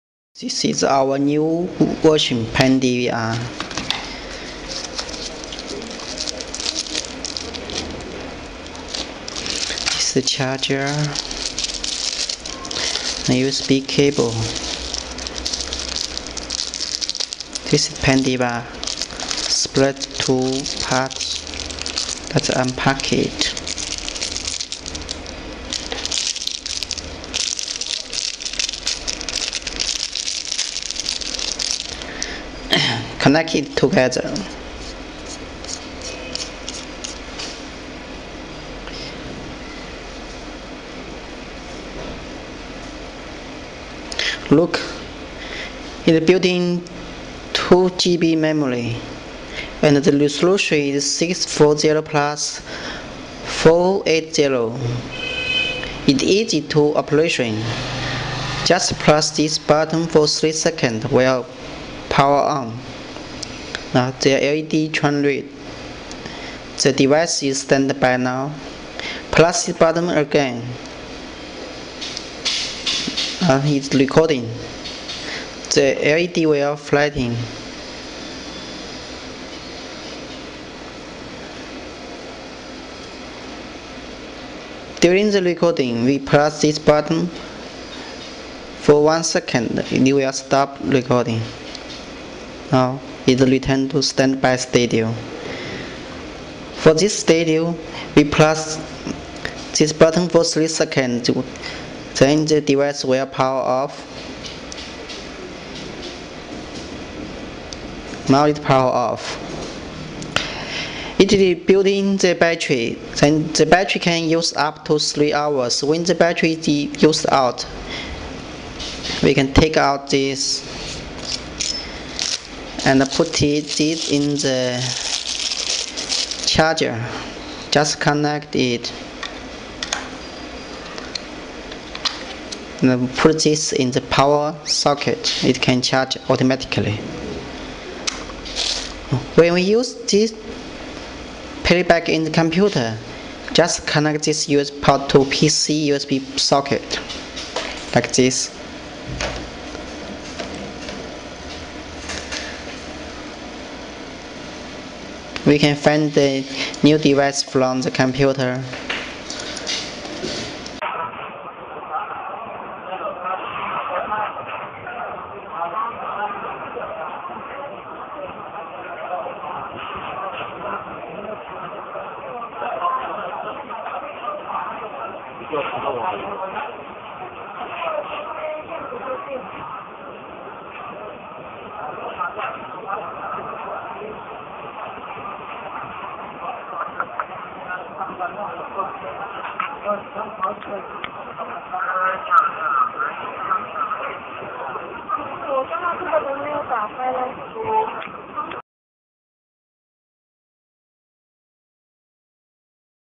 New Generation Pen DVR
Built in 2GB memory
Resolution: 640X480
Battery up to 3 hours
pinhole camera